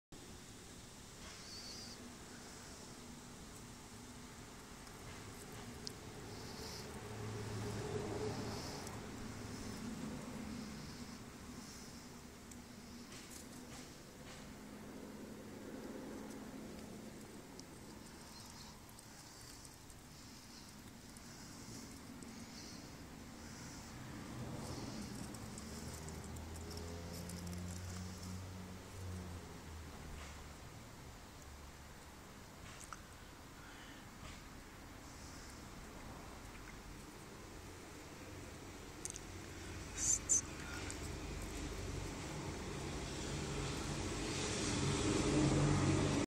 Tamia Rayé
Tamia qui mange des peanuts.